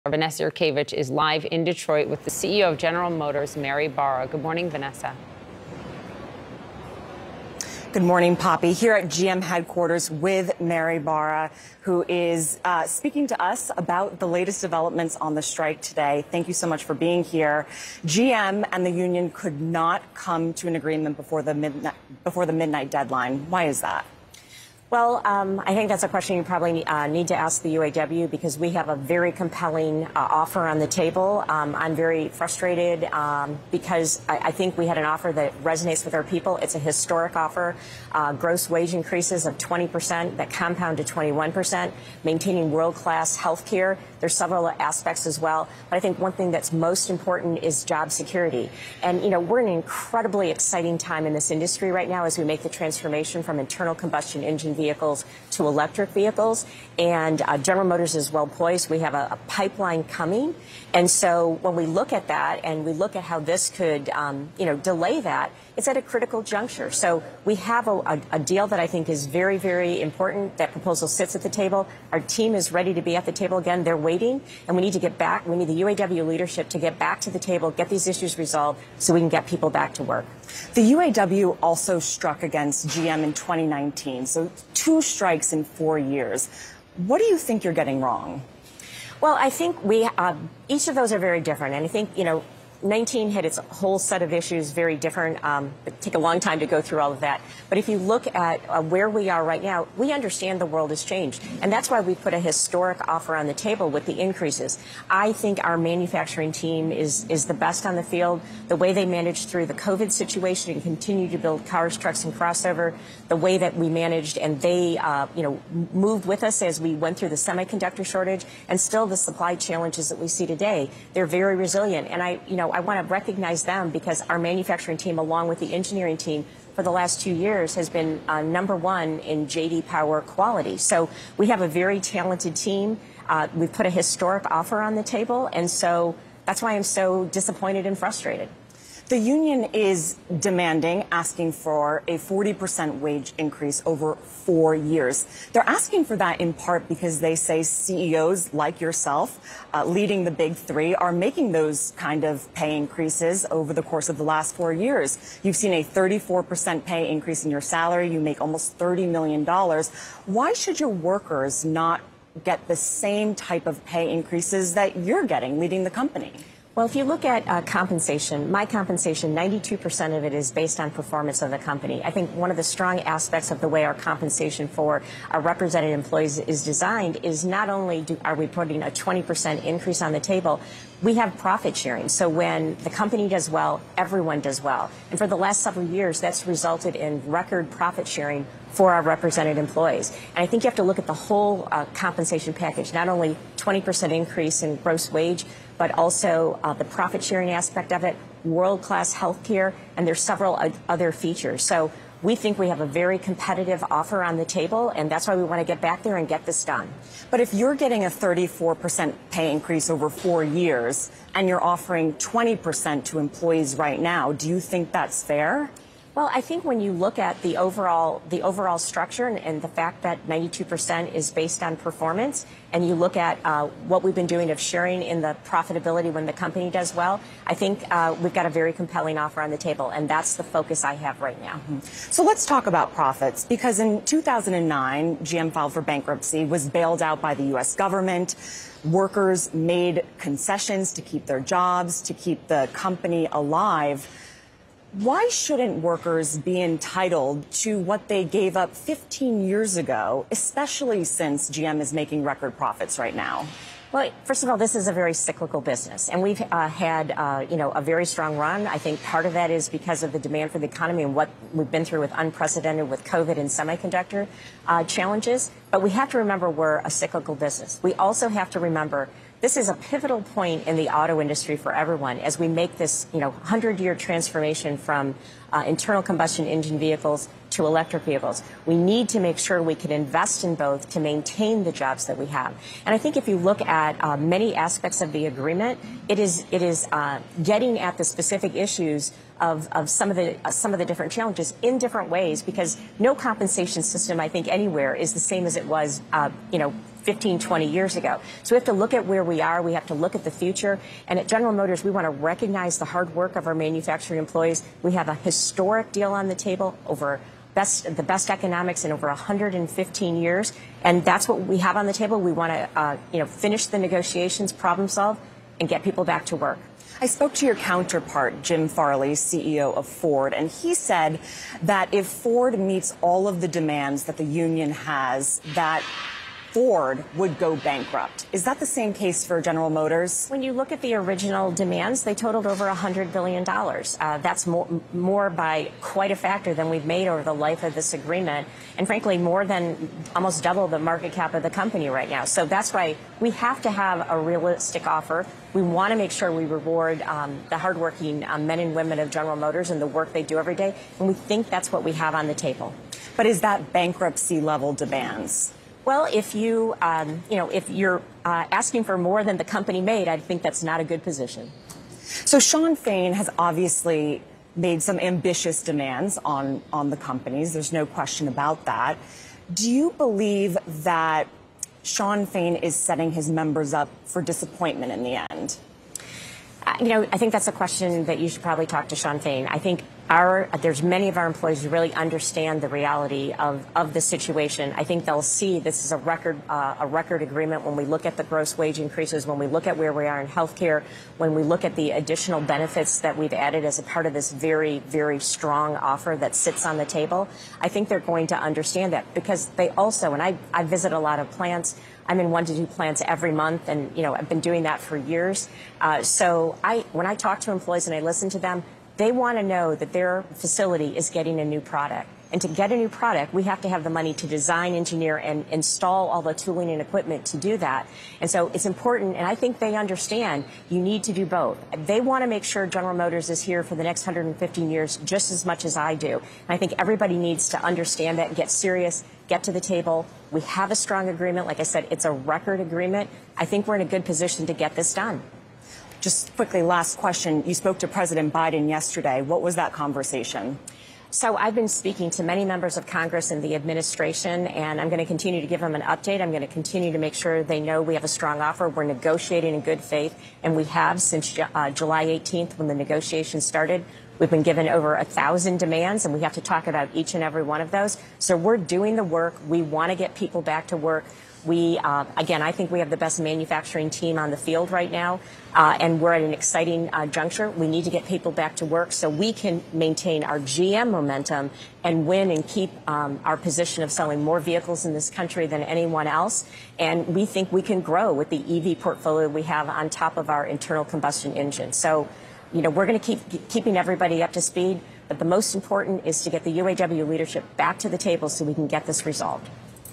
Reporter asks CEO about her nearly $29 million in compensation as workers strike
CNN's Vanessa Yurkevich interviews GM CEO Mary Barra as the United Auto Workers union is on strike against major US car manufacturers.